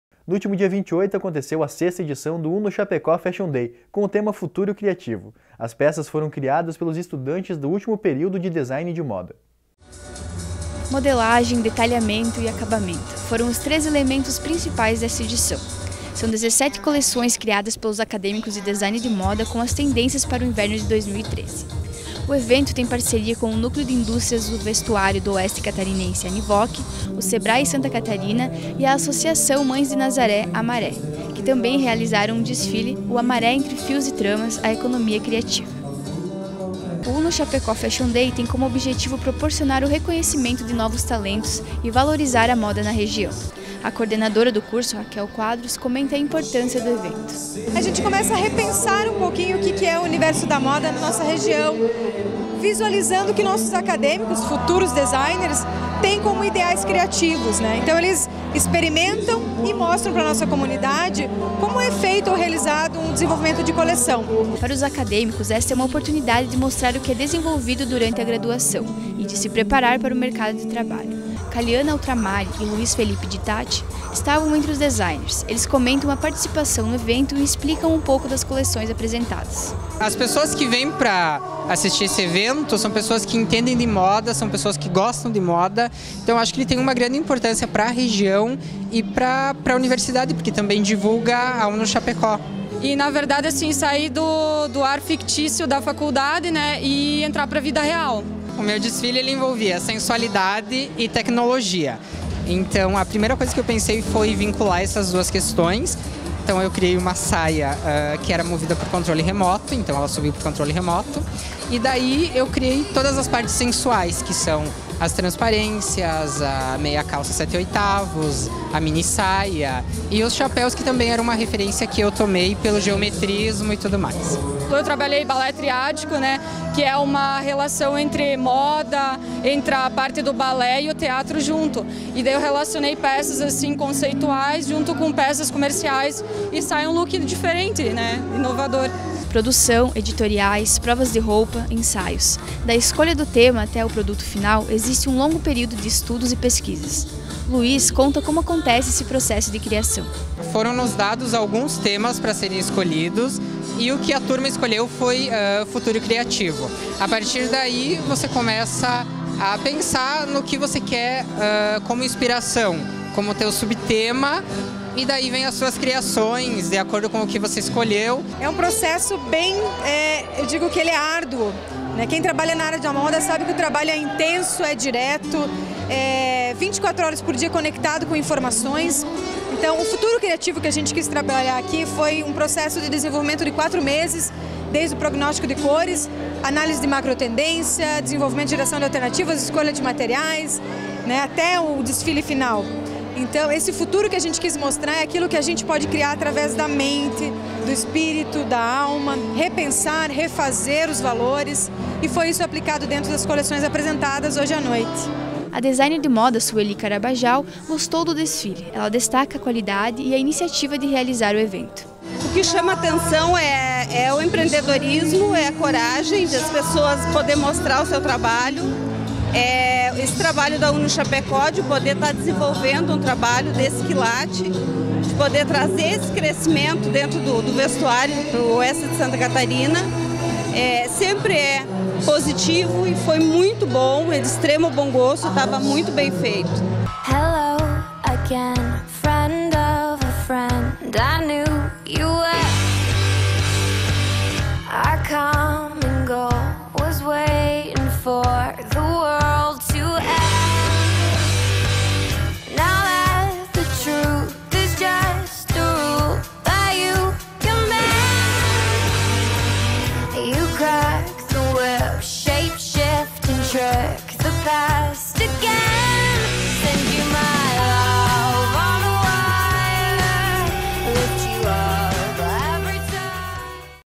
Universo Uno | Unochapecó Fashion Day 2012
Edição deste ano, contou com 17 coleções.